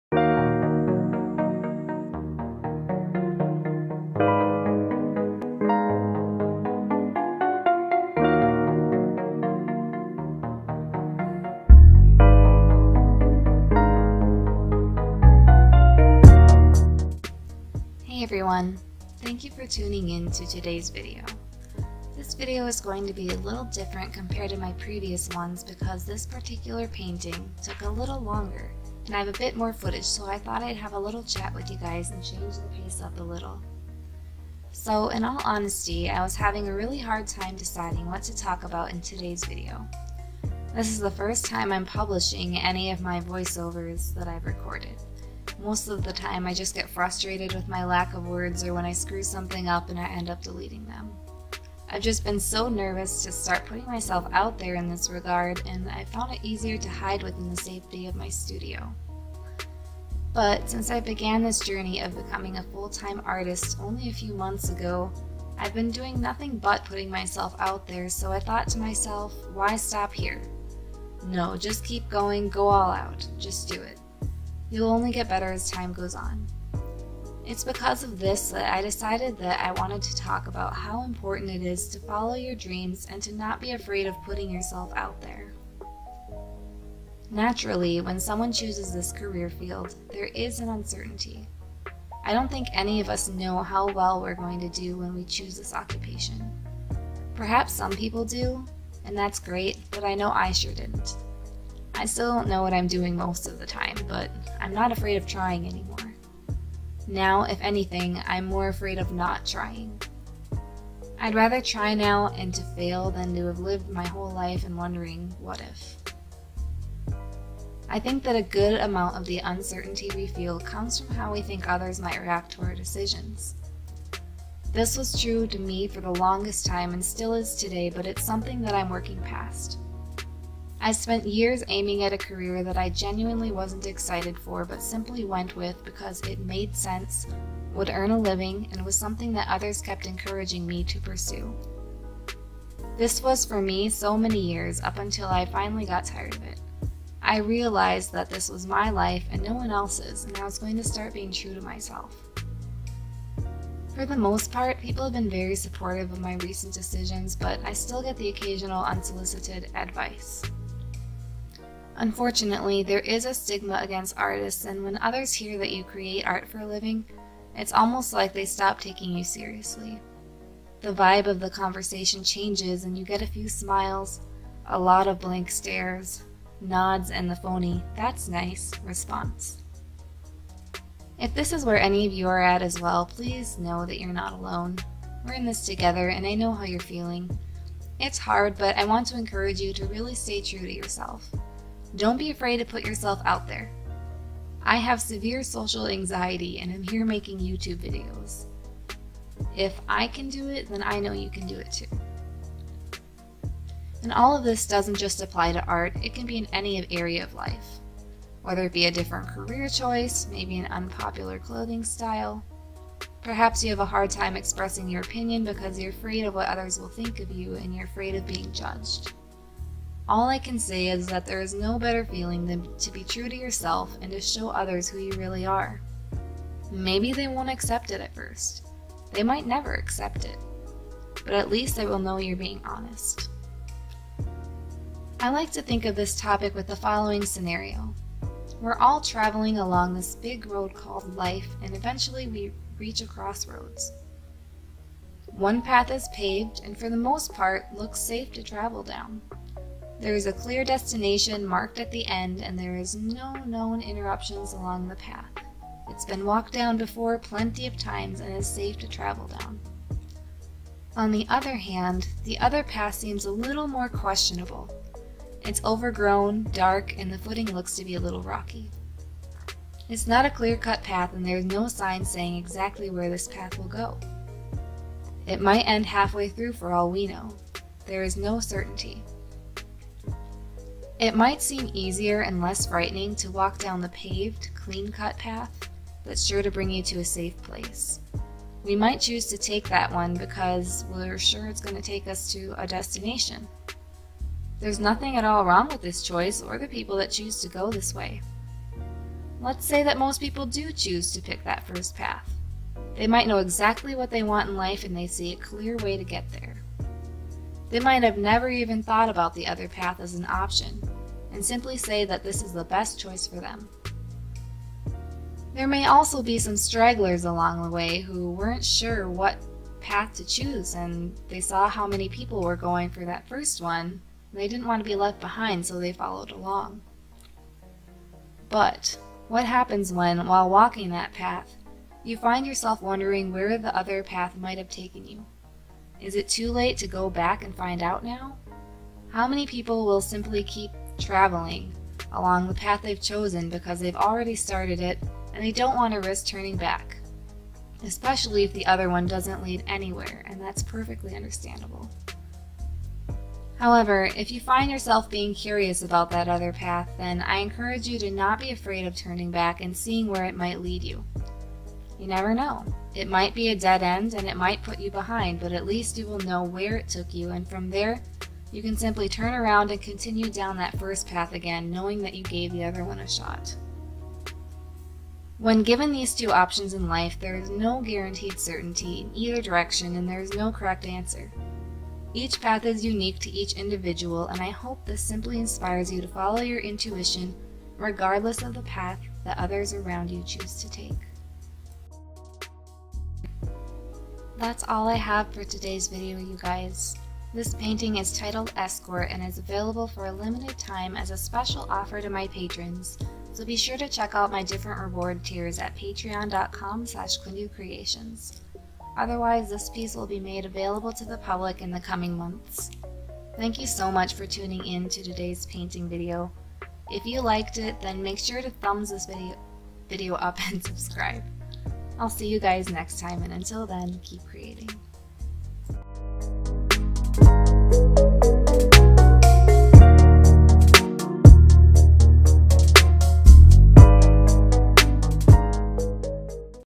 Dealing with Uncertainty. "Escort" Acrylic Painting Process
I hope you guys enjoy this week’s painting video! I know it’s really different from my last few - hopefully that’s a good thing.

I know that lighting is still an issue. 😞 I only have led lights in my studio and they’re terrible for filming! There is a lot of natural light present, but with the dark colors I’ve been using, a lot of parts still just seem fuzzy when picked up by the camera. All in all, I’m still looking for a good option for studio lights! If you have any suggestions, I’d love to read them down in the comments!